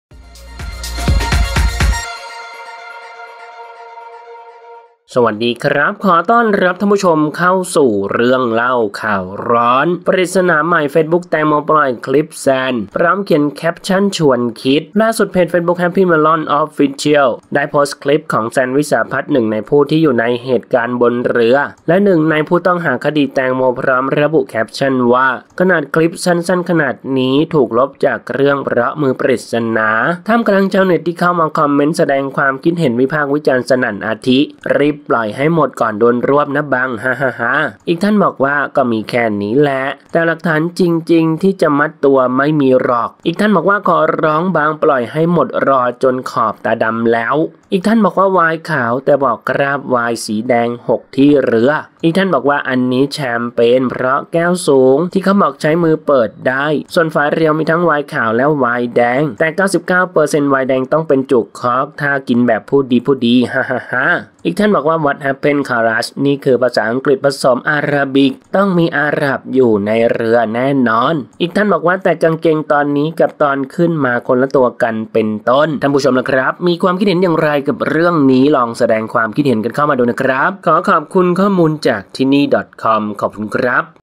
ปริศมาใหม่! เฟซบุ๊กแตงโมปล่อยคลิปแซน พร้อมแคปชั่นชวนคิด
แม้ก่อนหน้านี้ "บังแจ็ค" จะออกมาโพสต์ว่าได้ส่งของกลับคืนคุณแม่ของแตงโม นิดา แล้ว แต่ขณะเดียวเพจเฟซบุ๊ก หรือไอจีช่องทางโซเชียลมีเดียต่างๆ ที่เกี่ยวข้องกับแตงโมก็ยังไม่หยุดเคลื่อนไหว ออกมาโพสต์ภาพ คลิป อย่างต่อเนื่อง

เพราะล่าสุด เพจเฟซบุ๊ก Happy Melon official ก็ได้โพสต์คลิปของ แซน วิศาพัช 1ในผู้อยู่ในเหตุการณ์บนเรื่อ และ1ในผู้ต้องหาคดีแตงโม พร้อมแคปชั่นระบุว่า
.ขนาดคลิปสั้นสั้นขนาดนี้ถูกลบจากเครื่องเพราะมือปริศนา...